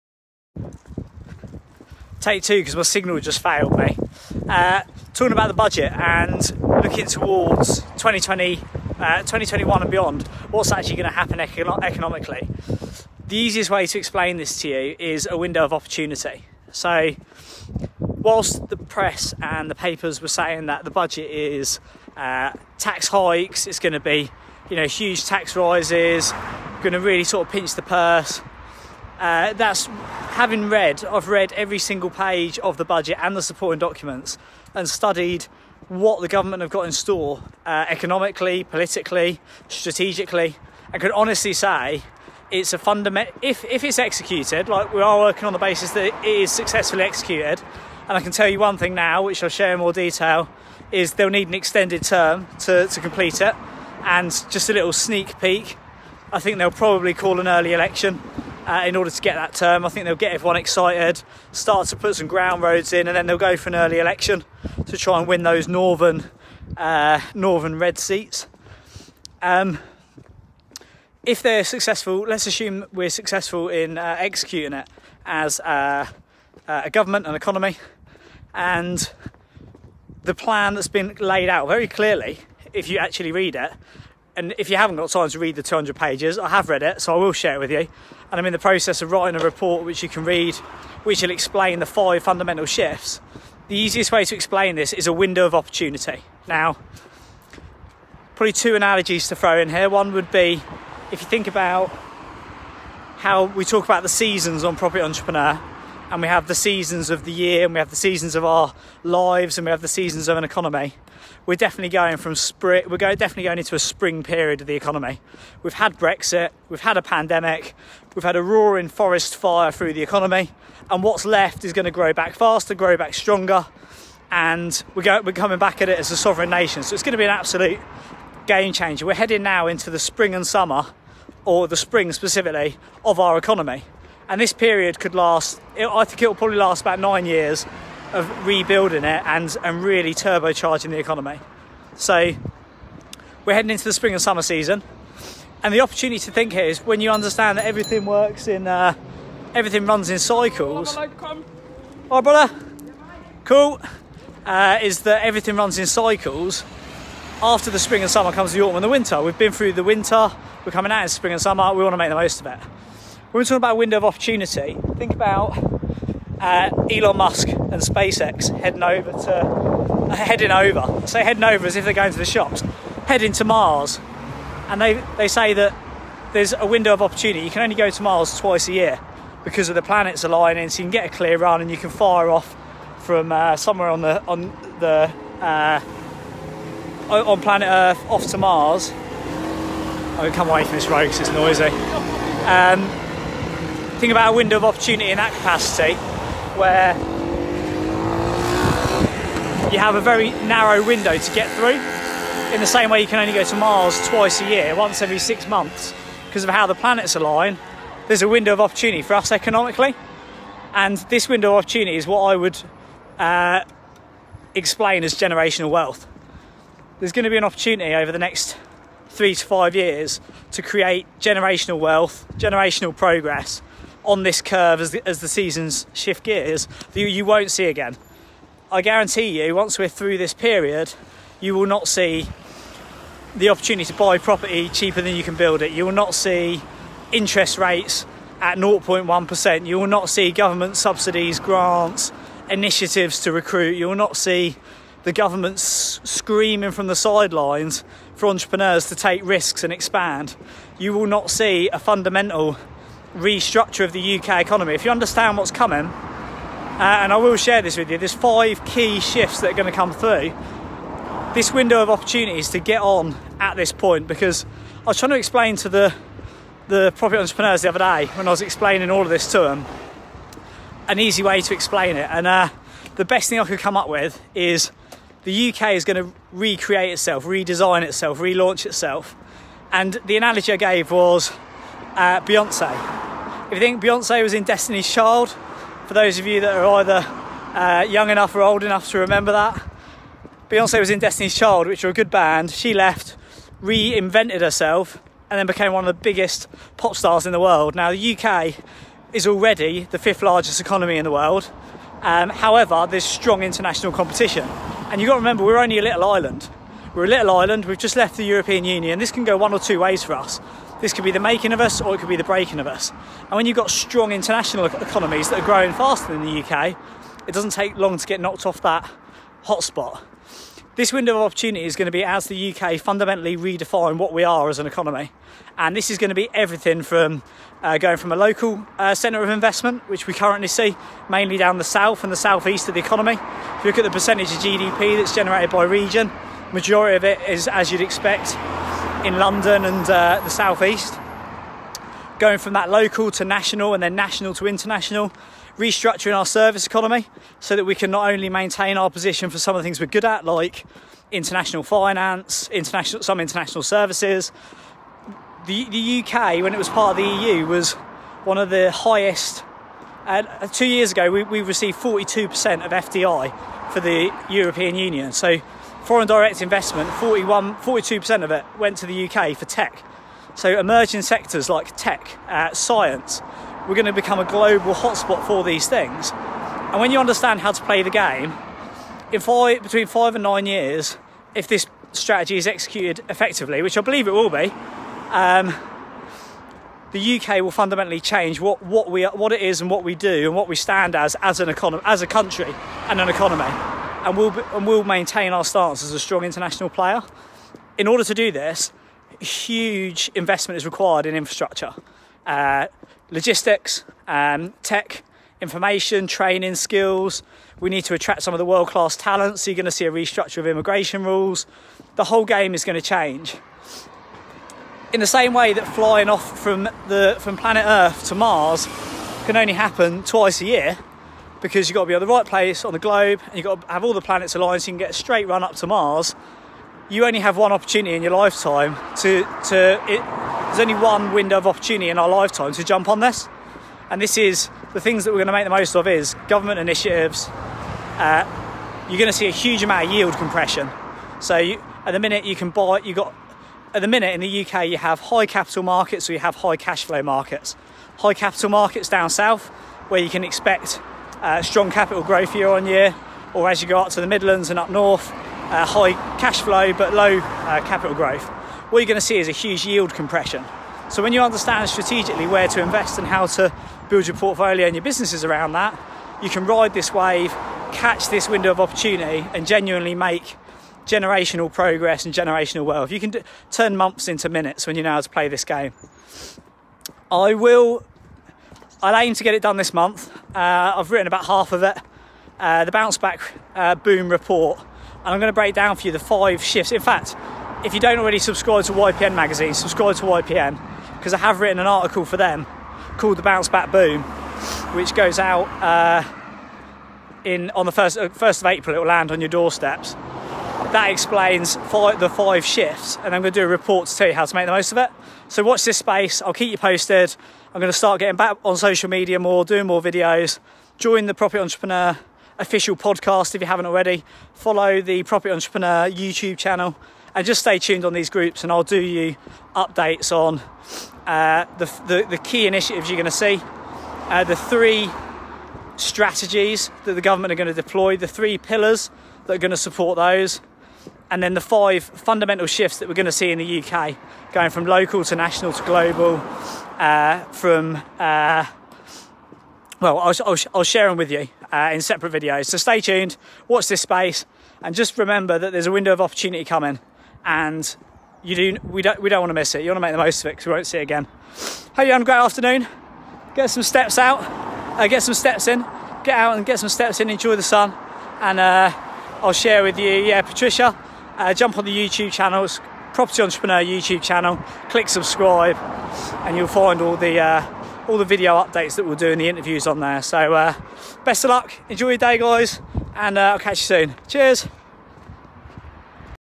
UK Bounce Back Boom 🌱☀️ The window of opportunity... Do not miss this | Property Entrepreneur
THE 2021 BUDGET REVIEW | Everything That You Need To Know | Property Entrepreneur

In this budget bounce back there is a huge opportunity to scale your business to the next level and make a positive impact in the market.

The 2021 budget facts of the united kingdom that most people don't know. I have a full copy of the budget. I have read all the facts and want to share them with you. I hope you will get some value.